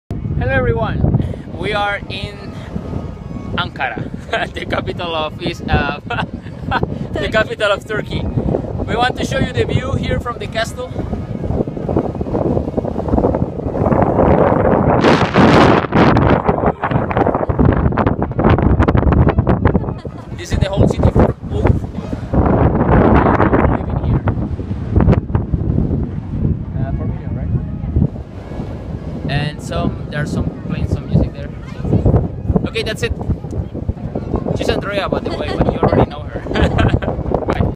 Ankara - Turkey (Turquía). Hello Everyone!
This is the Castle of Ankara (Ankara Kalesi). Enjoy the view, it is amazing!

If you have any question please don't hesitate to ask.